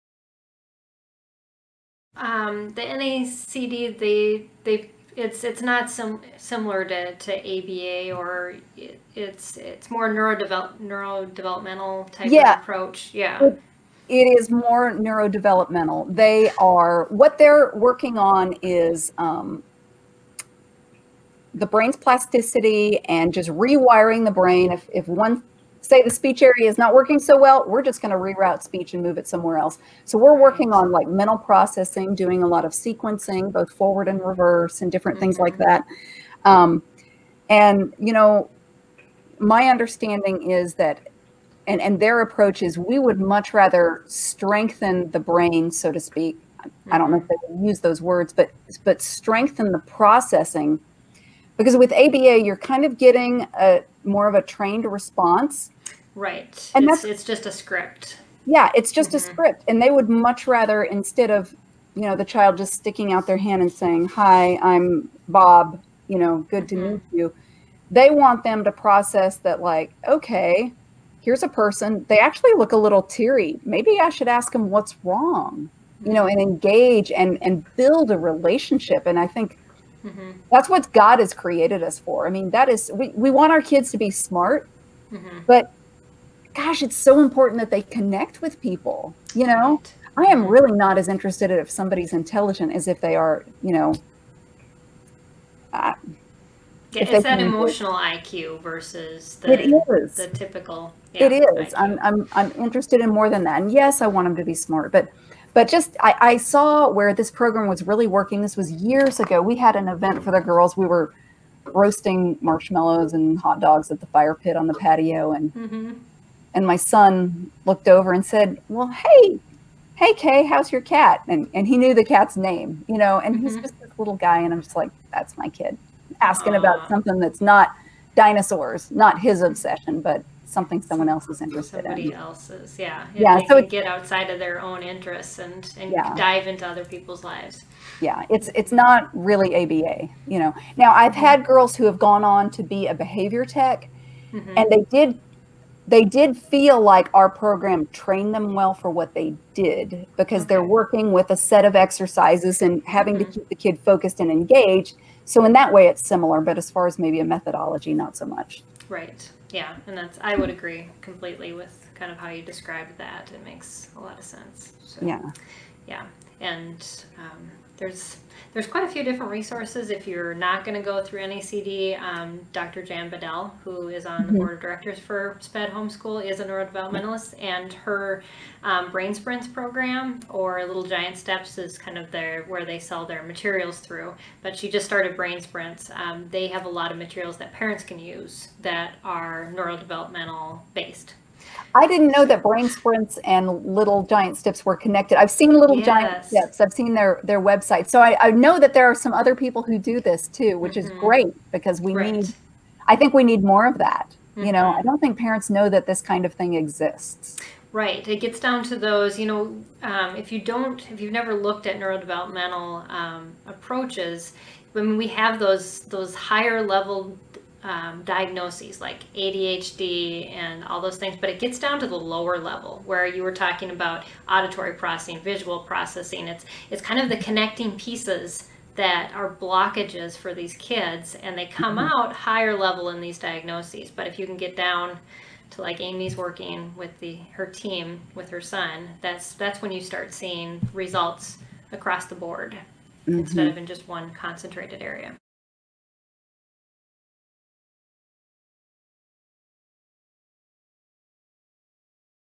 Differences Between Neurodevelopmental and Applied Behavior Approaches
Wondering what the difference is between a neurodevelopmental teaching approach and ABA, or an Applied Behavior Approach? Amy Gordon Stokes talks with us about these teaching approaches and how the neurodevelopmental approach works at the root level of learning issues over the ABA approach of scripting change.

To learn more about how Amy can help you develop a plan for being an administrator in your homeschool, check out her

To find out more about SPED Homeschool visit our